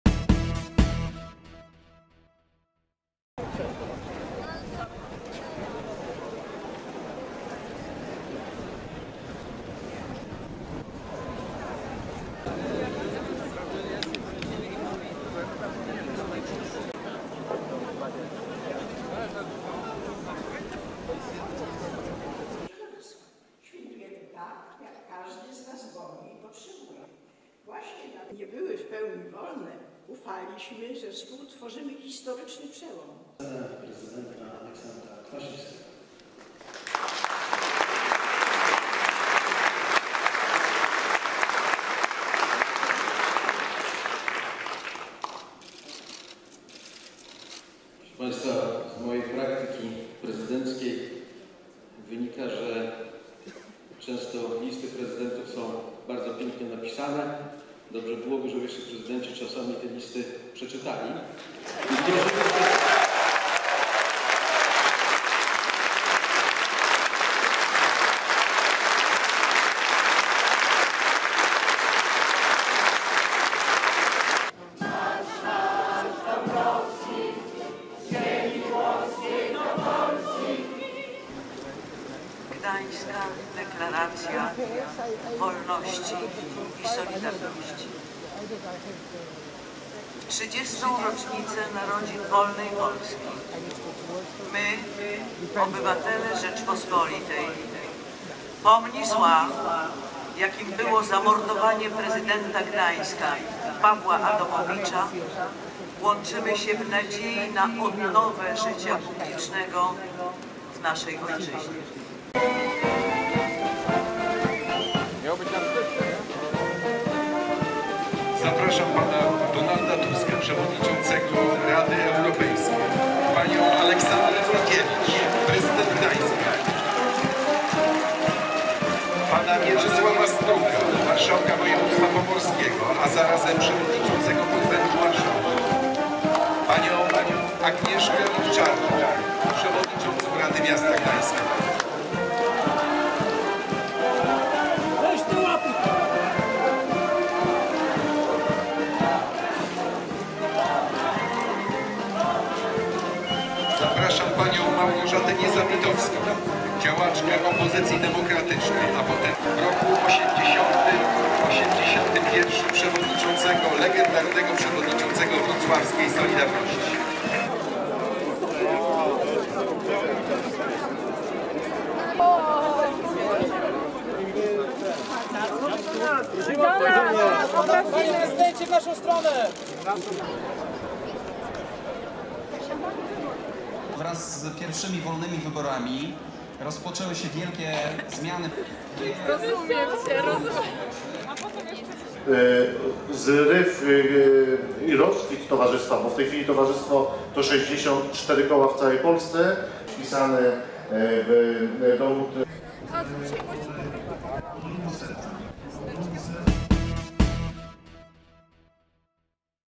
Gdańska Deklaracja Wolności i Solidarności z okazji 30. rocznicy wyborów 4 czerwca 1989
Dokładnie 30 lat po wyborach 4 czerwca 1989 r. przed ECS została podpisana Gdańska Deklaracja Wolności i Solidarności. We wtorek, 4 czerwca 2019 r. sygnowali ją m.in.: Lech Wałęsa, Aleksander Kwaśniewski, Bronisław Komorowski, Donald Tusk, Aleksandra Dulkiewicz, Mieczysław Struk oraz samorządowcy, działacze społeczni, młodzi gdańszczanie i politycy za całej Polski.